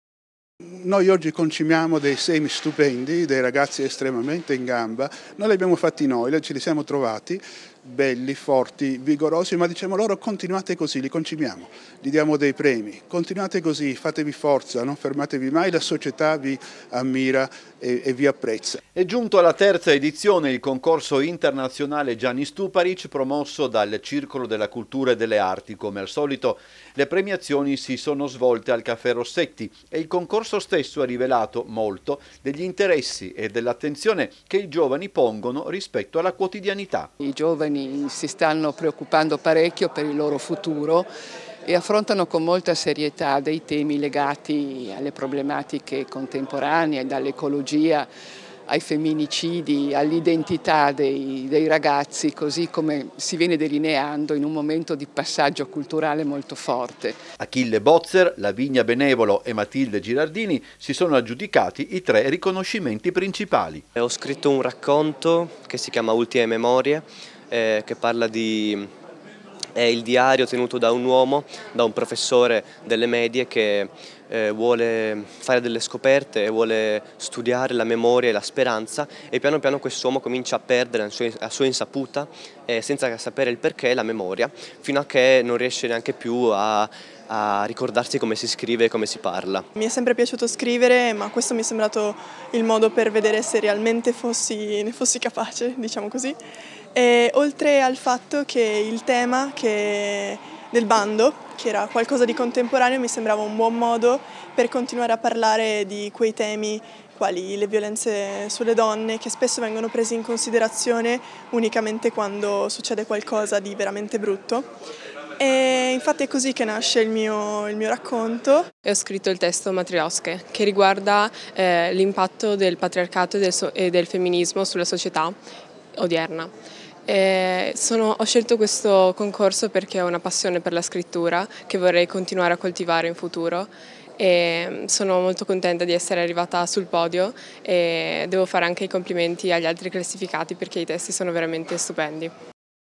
PREMIATI I VINCITORI DEL CONCORSO LETTERARIO DEL CCA DEDICATO AI GIOVANI | 05/05/2024
TG sera 05/05/2024 - TRIESTE - Giunto alla tera edizione, il concorso letterario indetto dal Circolo della Cultura e delle Arti ha visto ieri la premiazione degli elaborati sceklti dalla giuria. Ancora una volta, il concorso è stato molto partecipato. - Intervistati PIERO PAOLO BATTAGLIN (presidente CCA), CRISTINA BENUSSI (responsabile sezione letteratura cca), ACHILLE BOZZER (primo classificato), lavinia benevolo (seconda classificata), matilde girardini (terza classificata) (Servizio di Umberto Bosazzi)  - Segui Telequattro anche sul digitale terrestre! Visita il sito telequattro.medianordest.it